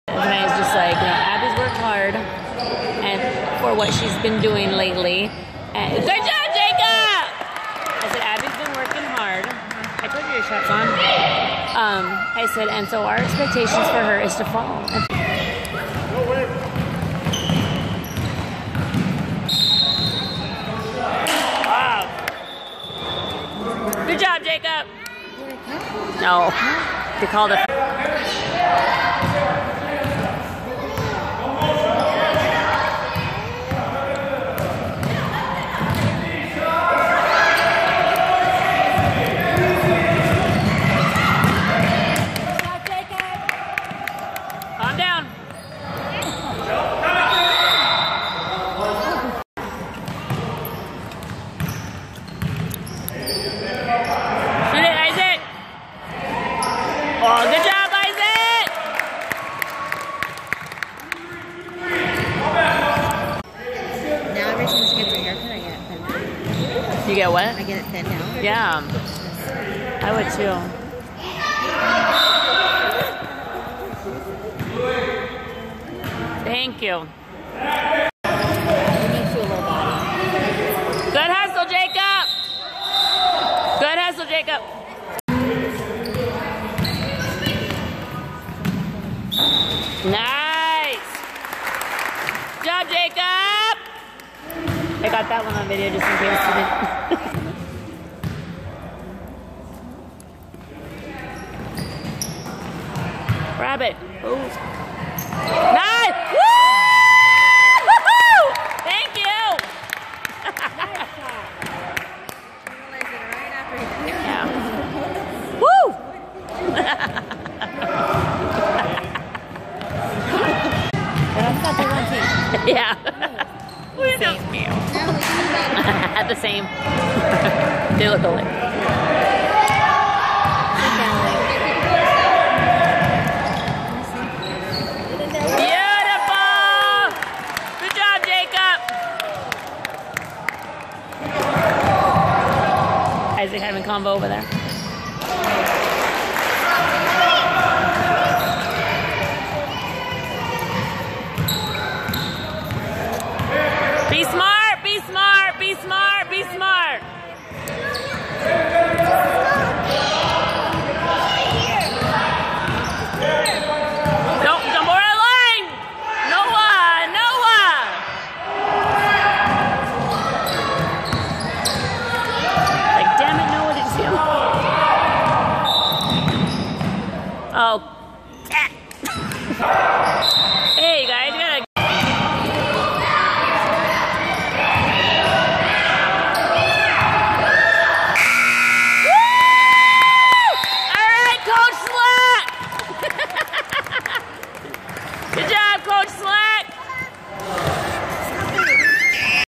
10/15/2016 YMCA Glendale/Peoria 5/6 Competitive Blue Team
So happy for this kiddos! They did so good together! They played so hard and they won! Go Wildcats!!!